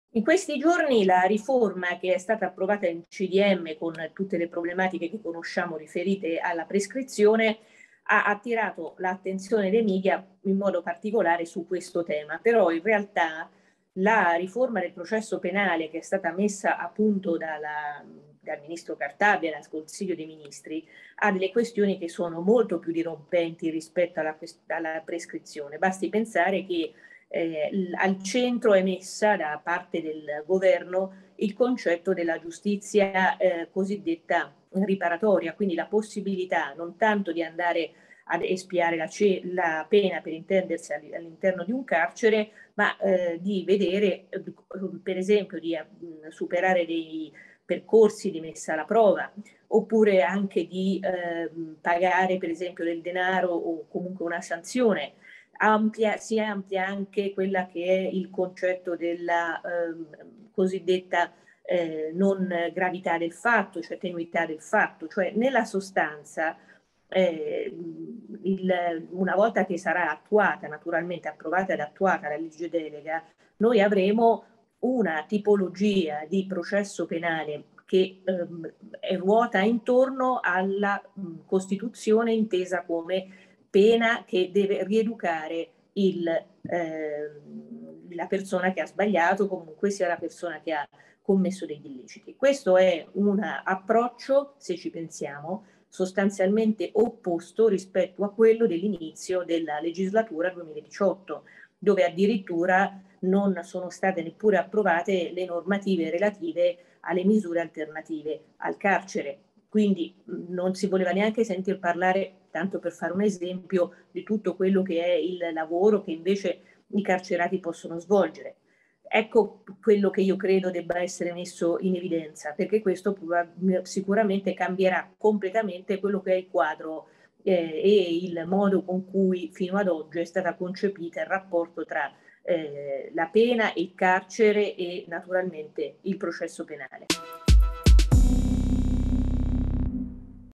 Giustizia, Modena (Fi): riforma apre a pene riparatorie e rieducative
Non solo prescrizione: per Fiammetta Modena (FI) “la riforma Cartabia cambia il modo in cui è stato concepito il rapporto tra la pena e il carcere”.